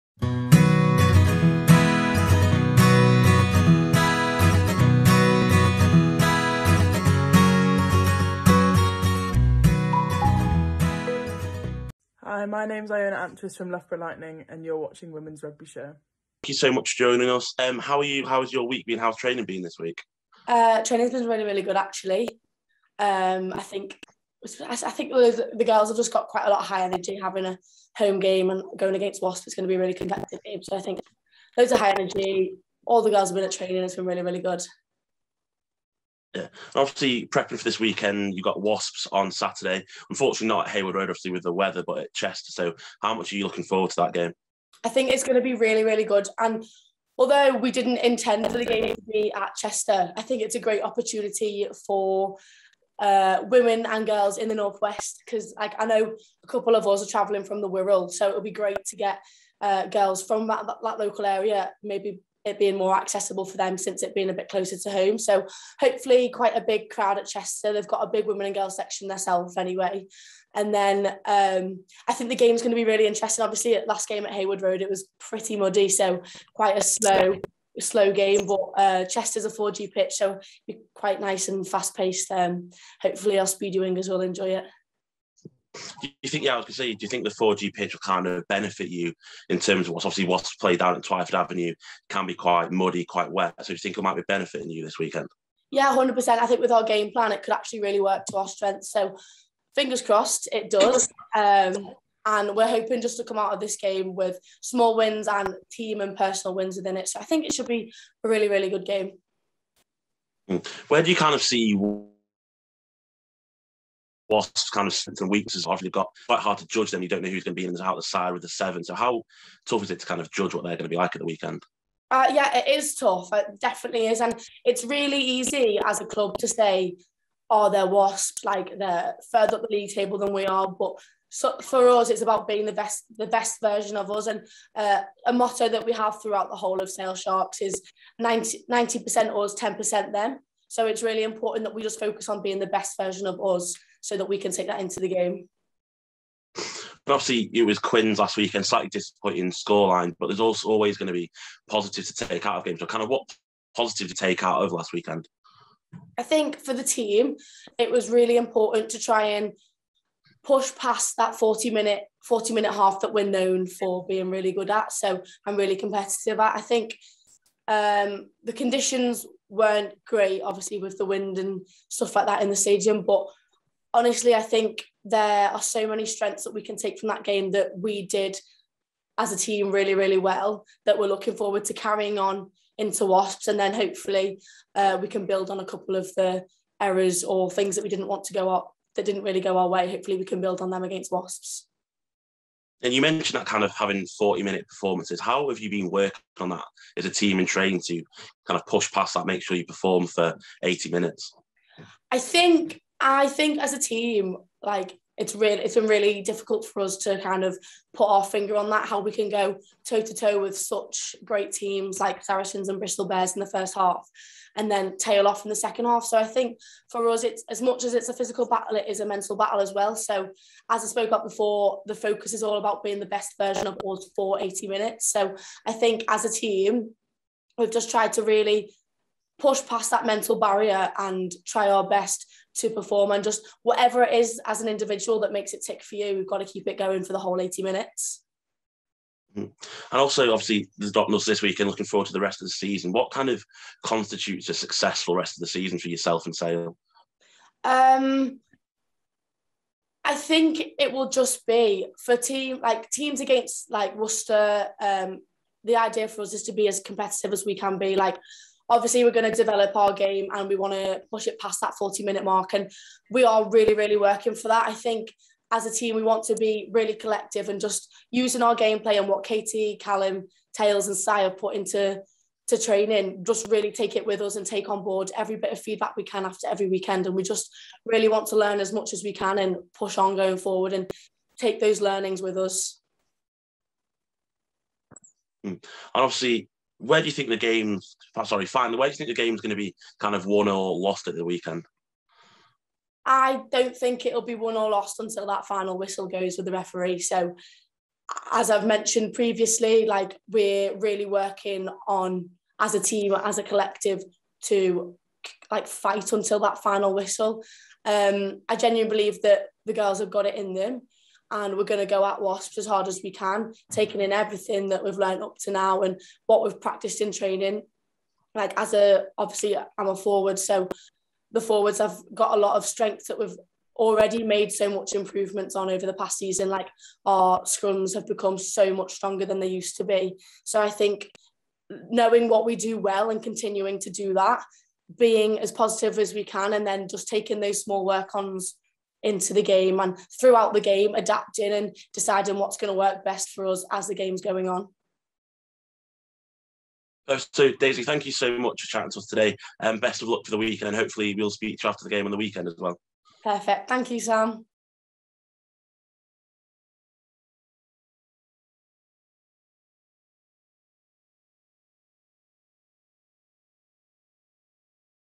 DAISY HIBBERT-JONES PREVIEWS WASPS CLASH l PLAYER PREVIEWS S2, EP 4
Ahead of our visit to Sale Sharks Women vs Wasps this weekend, Sam chatted to Sale back-row Daisy Hibbert-Jones to preview the match.